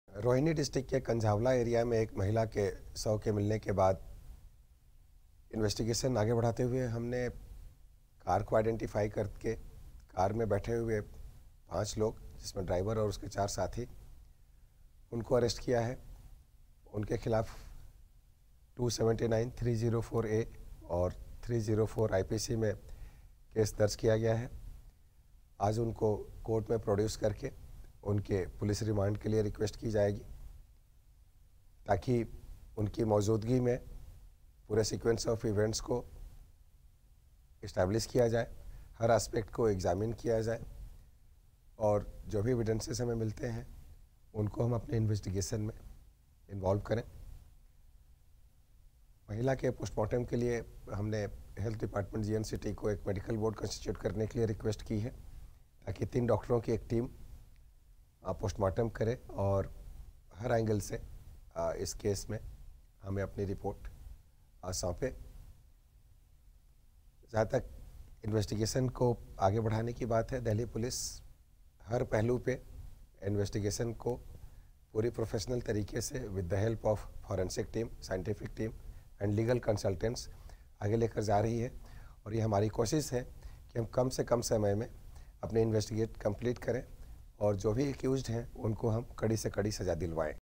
Kanjhawala Accident Case: DCP Outer ka bayan
Kanjhawala death case: Girl was not alone at time of accident, accused were returning from Murthal, more details emerge